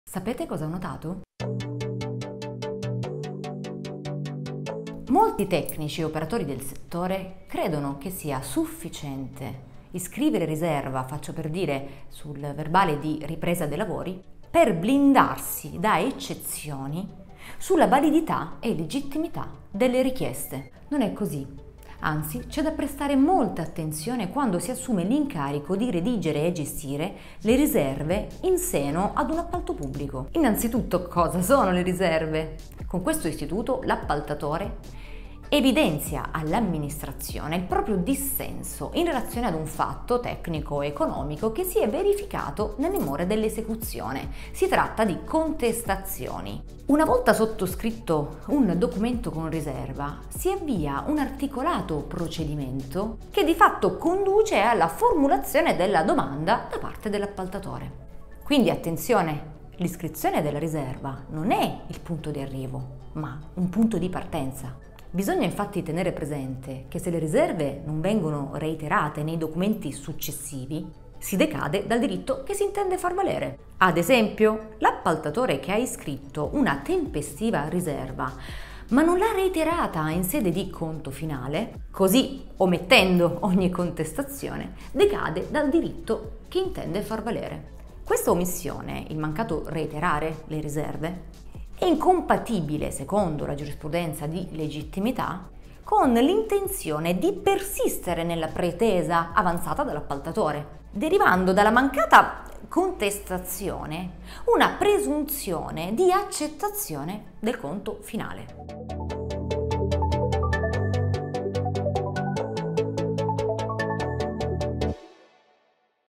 APPALTI Le riserve vanno reiterate sempre!
Molti tecnici e operatori del settore credono che sia sufficiente iscrivere riserva sul verbale di ripresa dei lavori per blindarsi da eccezioni sulla validità e legittimità delle richieste.

Non è cosi. Anzi, c’è da prestare molta attenzione quando si assume l’incarico di redigere e gestire le riserve in seno ad un appalto pubblico.

SEGUIMI QUI

Il presente materiale è per fini informativi e non costituisce consulenza legale